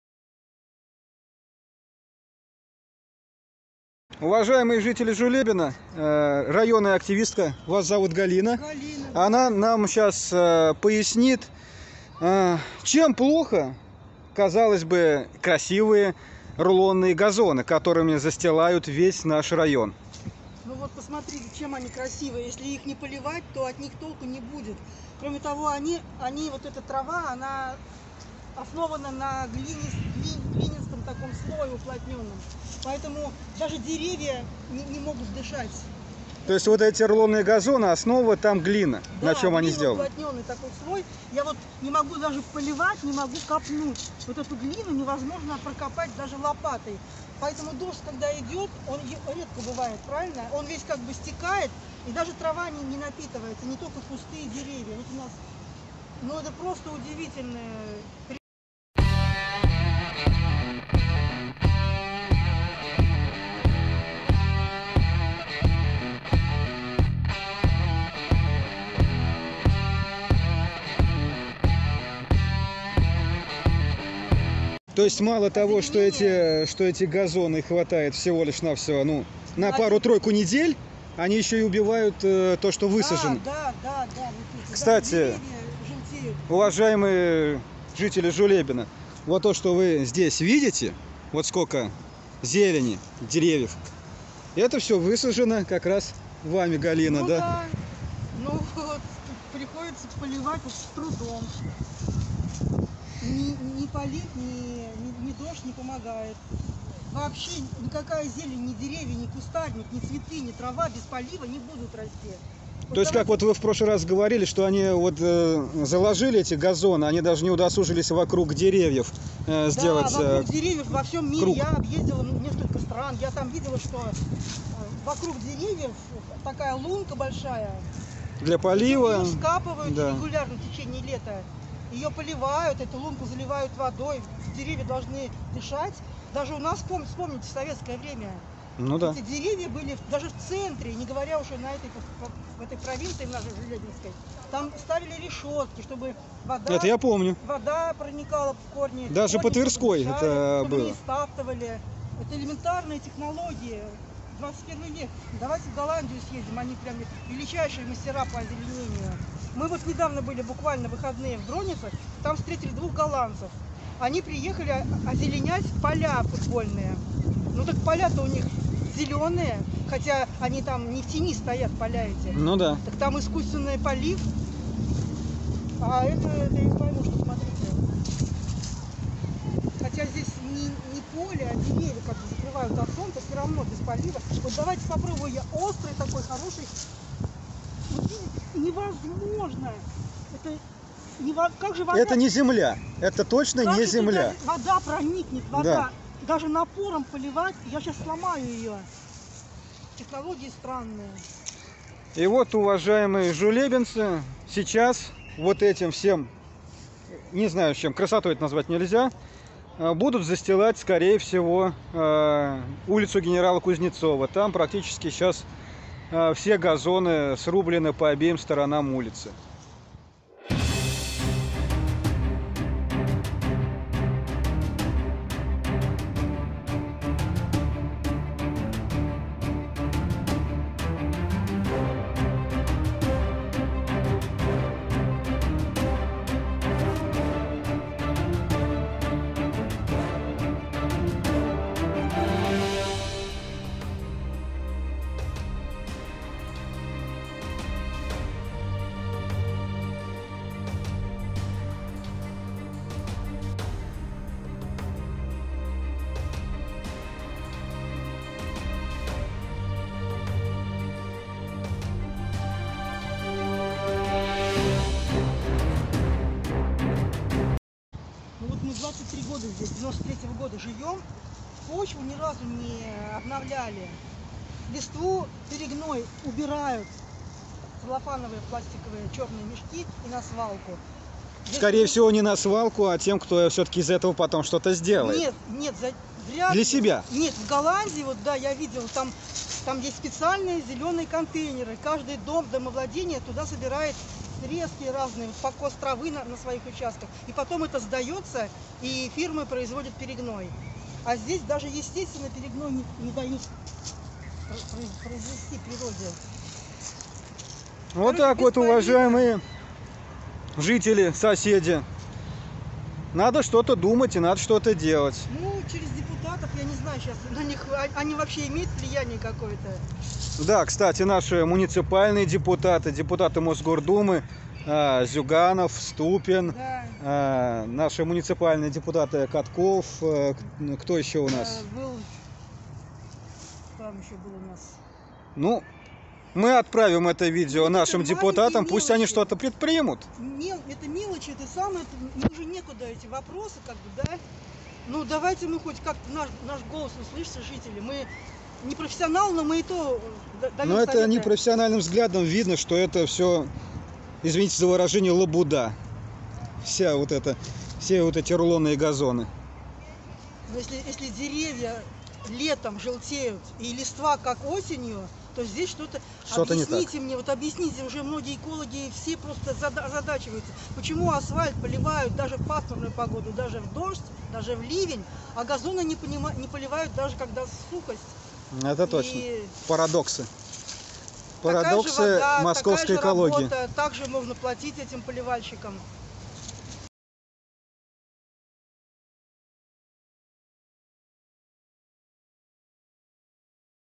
Жулебинская экоактивистка Галина про рулонные газоны
С недавних пор в городе повелась мода сдирать естественный верхний слой дёрна с газоном, и закатывать (в прямом смысле слова) "освободившееся пространство рулонными газонами. Сначала это как бы смотрится по итогу красиво, но всё самое интересное начинается потом...